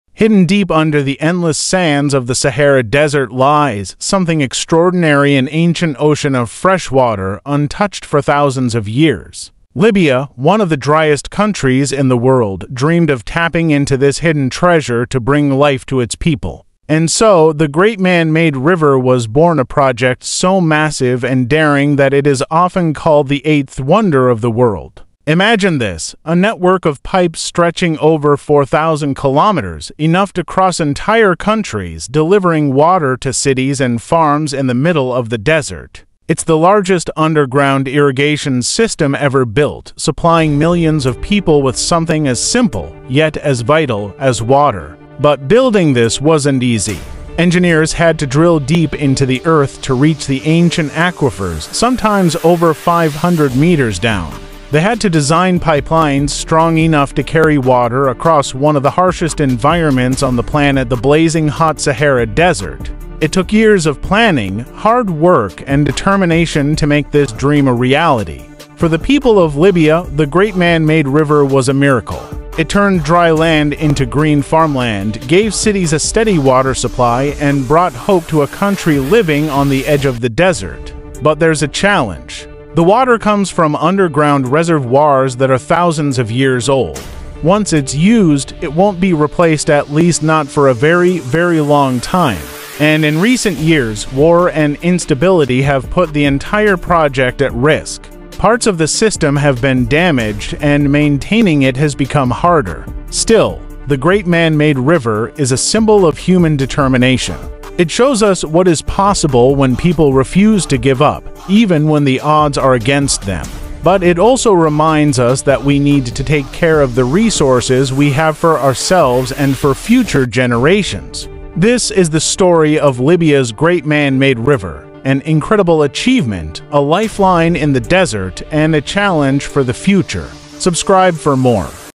Libya’s Great Man-Made River
Hidden beneath the golden sands of the Sahara Desert lies an engineering marvel that defies imagination. Libya’s Great Man-Made River, often called the Eighth Wonder of the World, is the largest underground water system ever built. This groundbreaking project brought life to one of the driest regions on Earth, transforming barren deserts into green farmland and supplying millions with fresh water.

Explore the incredible story of its creation, the challenges faced in its construction, and the ongoing battle to preserve this vital lifeline in a land shaped by nature and history. This documentary takes you deep into the heart of a modern miracle, showcasing the ingenuity, ambition, and resilience of human innovation.